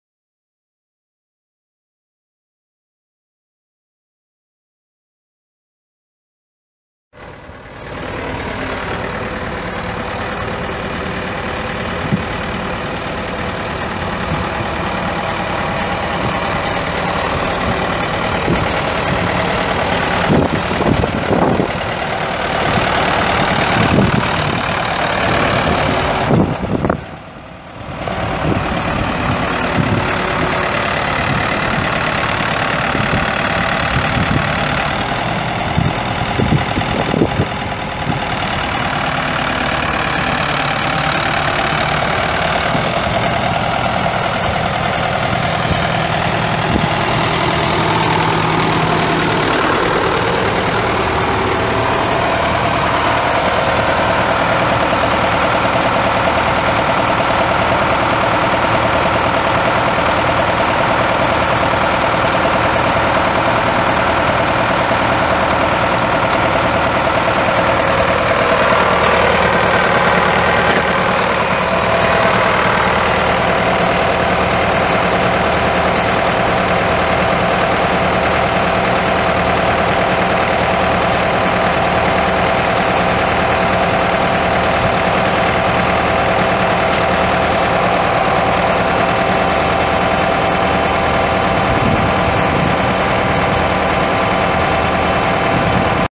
Ford 7710 MFWD tractor for sale | no-reserve Internet auction February 28, 2018
Ford 7710 MFWD tractor
5,273 hours on meter
Four cylinder diesel engine
Manual transmission
8F-4R gears
Enclosed cab
Three point
540 PTO
(4) rear hydraulic remote sets
13.6-28 front tires
18.4-38 rear tires
Serial BH76418
It's located in Meta, MO, and we're selling it to the highest bidder regardless of price.